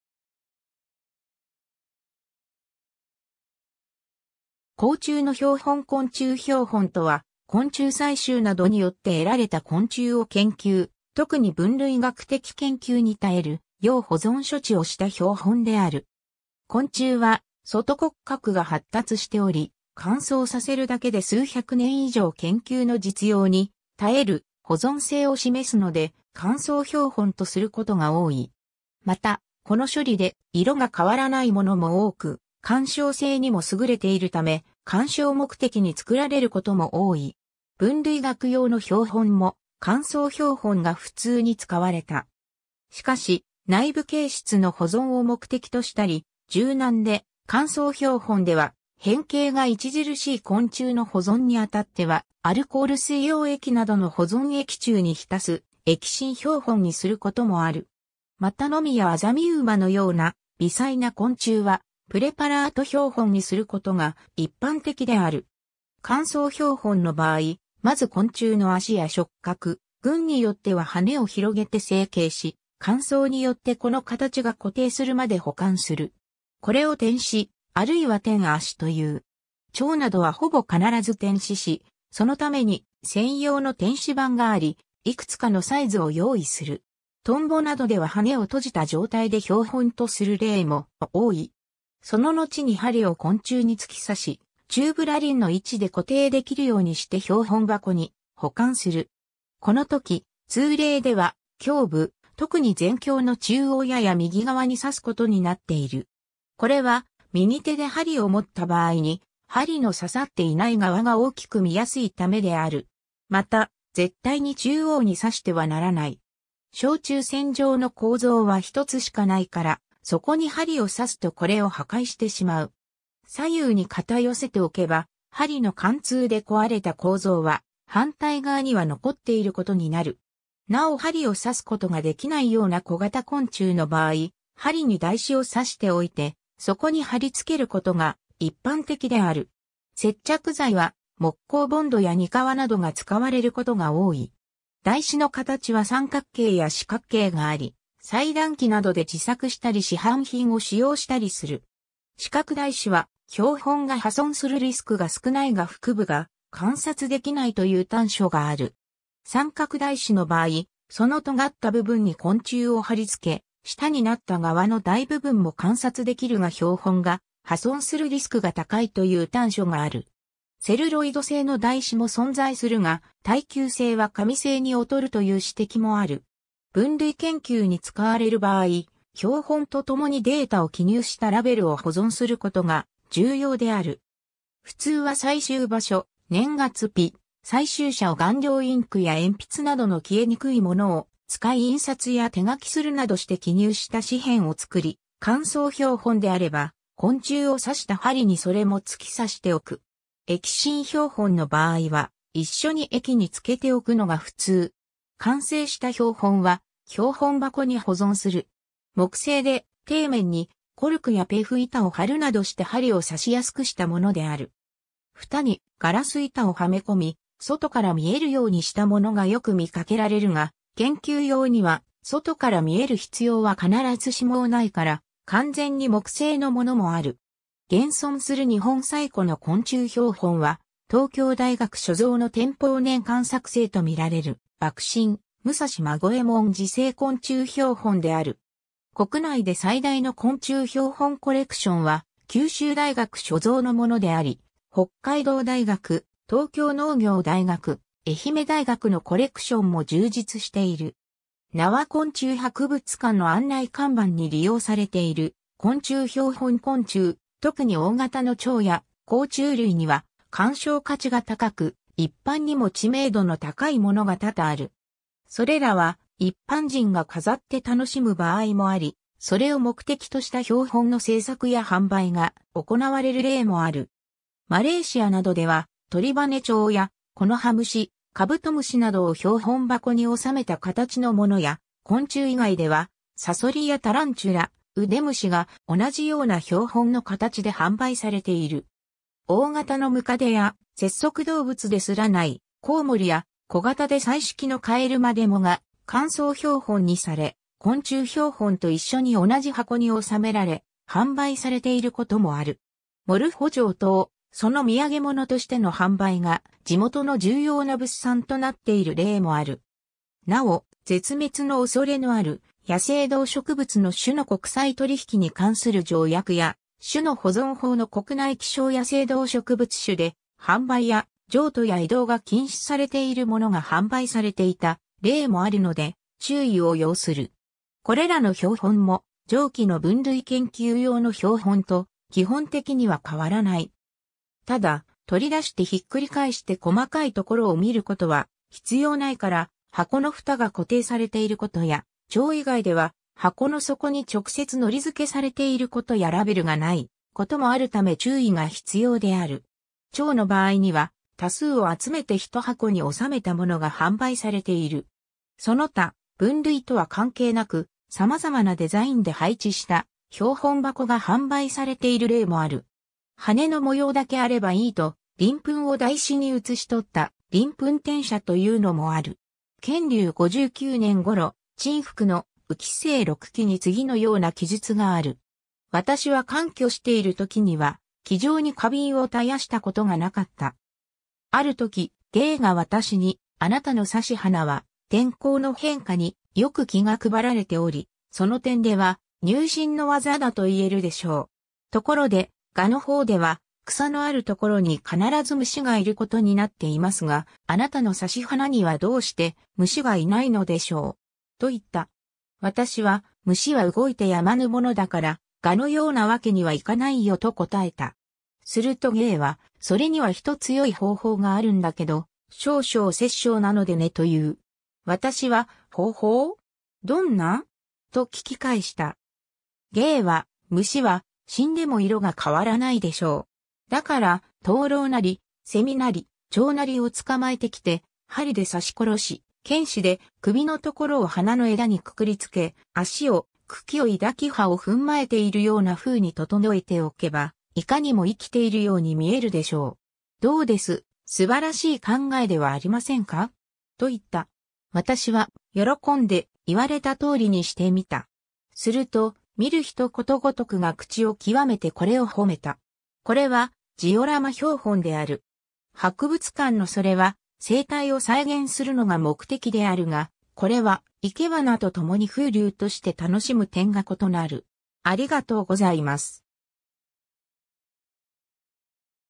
昆虫標本
甲虫の標本 昆虫標本（こんちゅうひょうほん）とは、昆虫採集などによって得られた昆虫を研究、特に分類学的研究に耐えるよう保存処置をした標本である。
昆虫は外骨格が発達しており、乾燥させるだけで数百年以上研究の実用に耐える保存性を示すので乾燥標本とすることが多い。
また、この処理で色が変わらないものも多く、観賞性にも優れているため、鑑賞目的に作られることも多い。
 分類学用の標本も乾燥標本が普通に使われた。
しかし、内部形質の保存を目的としたり、柔軟で乾燥標本では変形が著しい昆虫の保存に当たっては、アルコール水溶液などの保存液中にひたす液浸標本にすることもある。
またノミやアザミウマのような微細な昆虫はプレパラート標本にすることが一般的である。
 乾燥標本の場合、まず昆虫の足や触角、群によっては翅を広げて整形し、乾燥によってこの形が固定するまで保管する。
これを展翅、あるいは展足という。
チョウなどはほぼ必ず展翅し、そのために専用の展翅板があり、いくつかのサイズを用意する。
トンボなどでは翅を閉じた状態で標本とする例も多い。
 そののちに針を昆虫に突き刺し、宙ぶらりんの位置で固定できるようにして標本箱に保管する。
この時、通例では胸部、特に前胸の中央やや右側に刺すことになっている。
これは、右手で針を持った場合に針の刺さっていない側が大きく見やすいためである。
また、絶対に中央に刺してはならない。
正中線上の構造は一つしかないから、そこに針を刺すとこれを破壊してしまう。
左右に片寄せておけば、針の貫通で壊れた構造は反対側には残っていることになる。
 なお針を刺すことができないような小型昆虫の場合、針に台紙を刺しておいてそこに貼り付けることが一般的である。
接着剤は木工ボンドやニカワなどが使われることが多い。
台紙の形は三角形や四角形があり裁断機などで自作したり市販品を使用したりする。
四角台紙は標本が破損するリスクが少ないが腹部が観察できないという短所がある。
三角台紙の場合その尖った部分に昆虫を貼り付け、下になった側の大部分も観察できるが標本が破損するリスクが高いという短所がある。
セルロイド製の台紙も存在するが耐久性は紙製に劣るという指摘もある。
 分類研究に使われる場合、標本と共にデータを記入したラベルを保存することが重要である。
普通は採集場所、年月日、採集者を顔料インクや鉛筆などの消えにくいものを使い印刷や手書きするなどして記入した紙片(中性紙が最もよいとされる)を作り、乾燥標本であれば昆虫を刺した針にそれも突き刺しておく。
液浸標本の場合は一緒に液に浸けておくのが普通。
 完成した標本は、標本箱に保存する。
木製で、底面にコルクやペフ板を張るなどして針を刺しやすくしたものである。
蓋にガラス板をはめ込み、外から見えるようにしたものがよく見かけられるが、研究用には外から見える必要は必ずしもないから、完全に木製のものもある。
 現存する日本最古の昆虫標本は東京大学所蔵の天保年間作製とみられる「幕臣武蔵孫右衛門自製昆虫標本」である。
国内で最大の昆虫標本コレクションは九州大学所蔵のものであり、北海道大学、東京農業大学、愛媛大学のコレクションも充実している。
 名和昆虫博物館の案内看板に利用されている昆虫標本 （金華山ロープウェーの金華山麓駅にて） 昆虫、特に大型の蝶や甲虫類には鑑賞価値が高く、一般にも知名度の高いものが多々ある。
それらは一般人が飾って楽しむ場合もあり、それを目的とした標本の制作や販売が行われる例もある。
マレーシアなどではトリバネチョウやコノハムシ、カブトムシなどを標本箱に納めた形のものや、昆虫以外ではサソリやタランチュラ,ウデムシが同じような標本の形で販売されている（なお、一般的にクモ類は死後腹部が収縮してしまうため、乾燥標本には向かない）。
大型のムカデや、節足動物ですらない、コウモリや、小型で彩色のカエルまでもが、乾燥標本にされ、昆虫標本と一緒に同じ箱に収められ販売されている事もある。
モルフォチョウ等、その土産物としての販売が地元の重要な物産となっている例もある。
なお、絶滅のおそれのある野生動植物の種の国際取引に関する条約（ワシントン条約）や種の保存法の国内希少野生動植物種で販売や譲渡や移動が禁止されているものが販売されていた例もあるので、注意を要する。
 これらの標本も、上記の分類研究用の標本と基本的には変わらない。
ただ、取り出してひっくり返して細かいところを見ることは必要ないから、箱のふたが固定されていることやチョ...